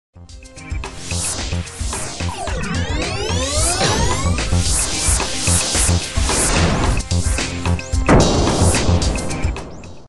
istyle- title
This is a title animation done for an Episode.
Done Entirely in 3DS MAX , Digital fusion & Adobe Priemere